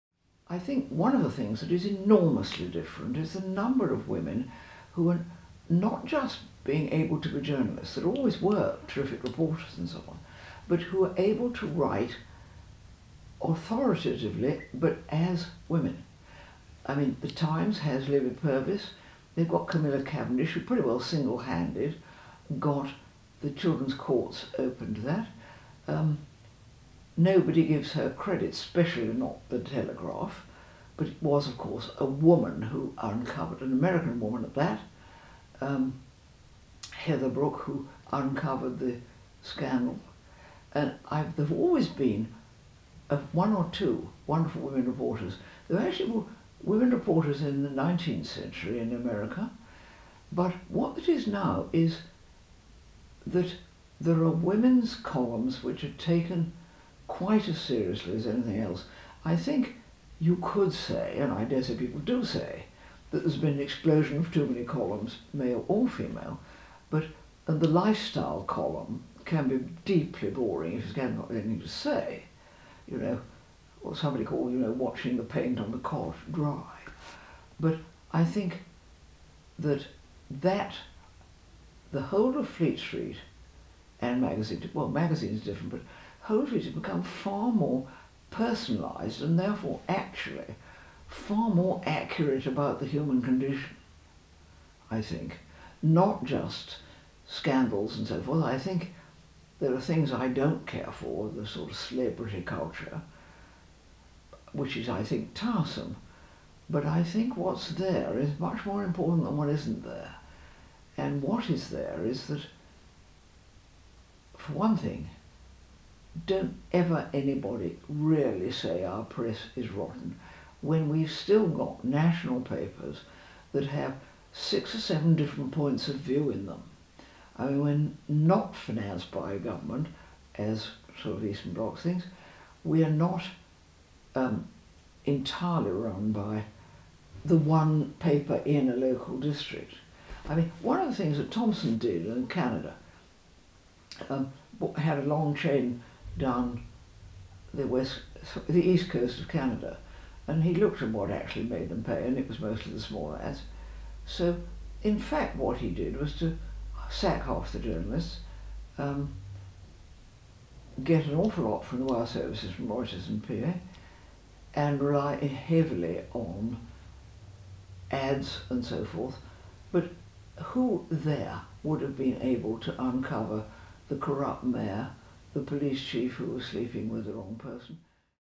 Katharine Whitehorn - Writing authoritatively as women (21/33)
A distinguished British journalist, Katharine Whitehorn was one of the first women columnists for the Observer and was their star columnist for over 30 years. Educated at Cambridge, she is recognised as someone who has transformed 20th century women's journalism. [Listener: Bob Bee]

TRANSCRIPT: I think one of the things that is enormously different is the number of women who are not just being able to be journalists... there always were terrific reporters and so on... but who are able to write authoritatively, but as women. I mean, ‘The Times’ has Libby Purvis. They’ve got Camilla Cavendish, who pretty well singlehanded got the children’s courts open to that. Nobody gives her credit, especially not ‘The Telegraph’.  But it was of course a woman who uncovered... an American woman at that, Heather Brooke, who uncovered the scandal. And I... there’ve always been one or two wonderful women reporters.

There actually were women reporters in the 19th century in America, but what it is now is that there are women’s columns which are taken quite as seriously as anything else. I think you could say, and I dare say people do say, that there’s been an explosion of too many columns, male or female, but the lifestyle column can be deeply boring if you haven’t got anything to say. You know? What somebody, you know, called watching the paint on the cot dry. But I think that... that the whole of Fleet Street and magazines... well, magazines are different... but the whole of Fleet Street has become far more personalised, and therefore actually far more accurate about the human condition, I think. Not just scandals and so forth.

I think there are things I don’t care for, the sort of celebrity culture, which is, I think, tiresome. But I think what’s there is much more important than what isn’t there. And what is there is that for one thing, don’t ever anybody really say our press is rotten when we’ve still got national papers that have six or seven different points of view in them. I mean, we’re not financed by government as, sort of, Eastern Bloc things. We’re not entirely run by the one paper in a local district.

I mean, one of the things that Thomson did in Canada... had a long chain down the west... the east coast of Canada. And he looked at what actually made them pay and it was mostly the small ads. So in fact what he did was to sack half the journalists, get an awful lot from the wire services, from Reuters and PA, and rely heavily on ads and so forth. But who there would have been able to uncover the corrupt mayor, the police chief who was sleeping with the wrong person?